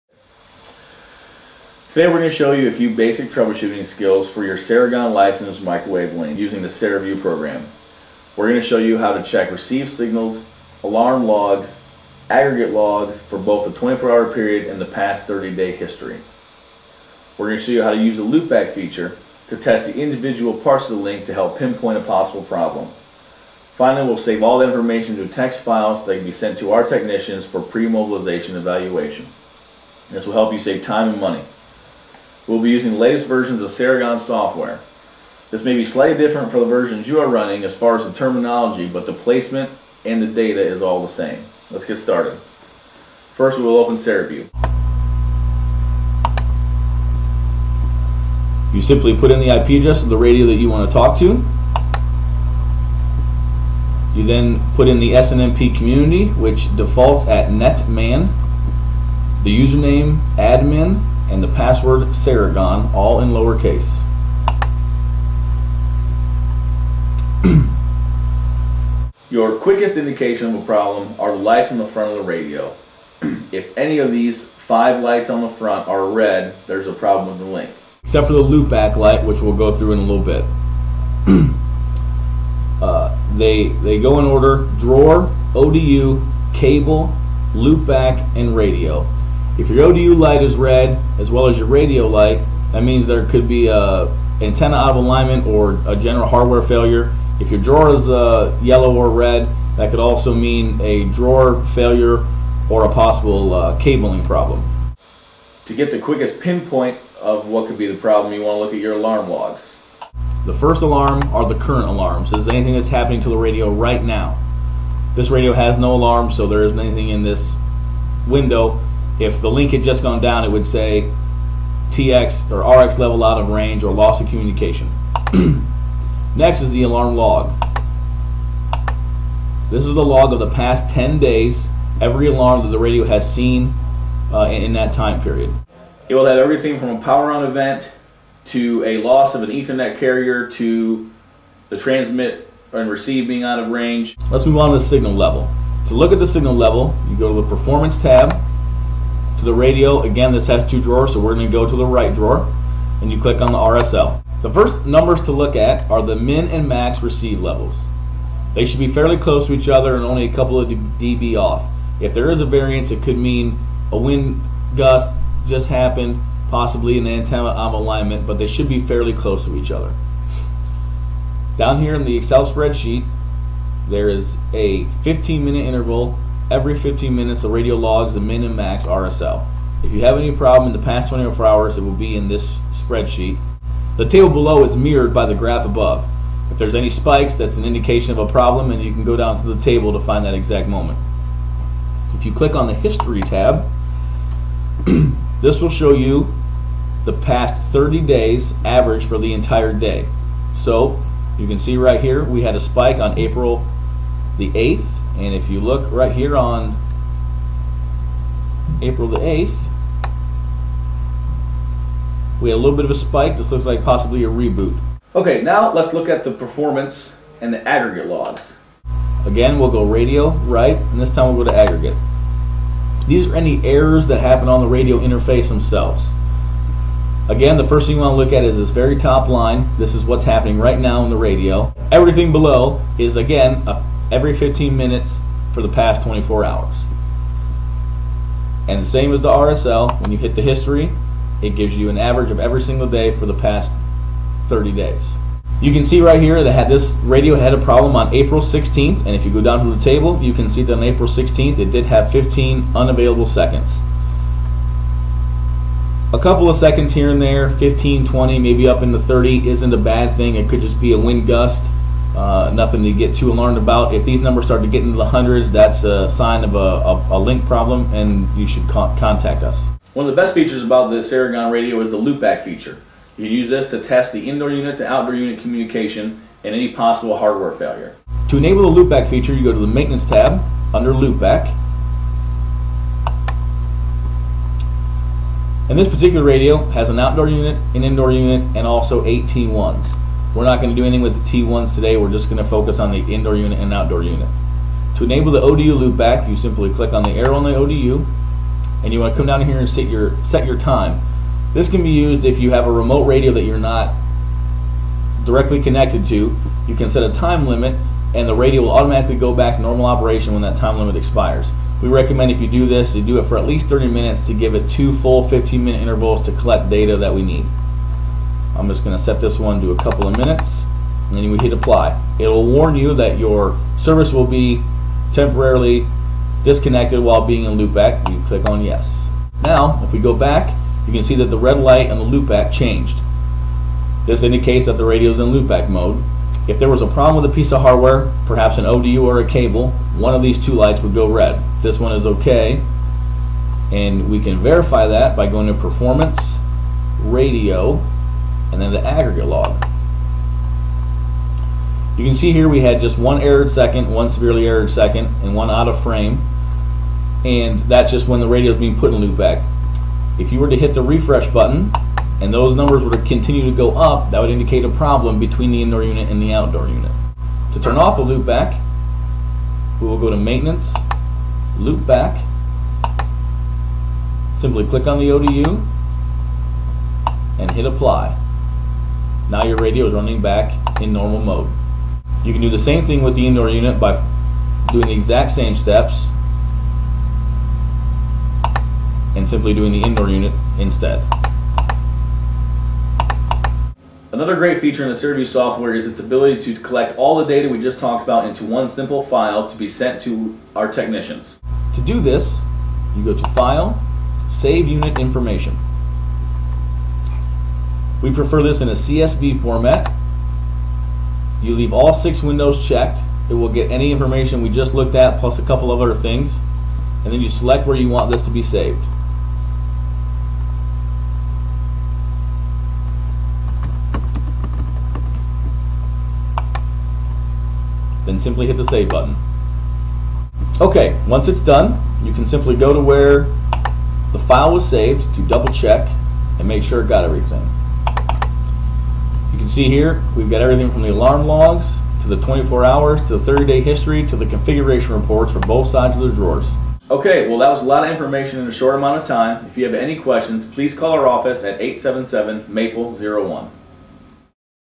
MapleNet Wireless Ceragon Video
A quick trouble shooting guide for your Ceragon Microwave Link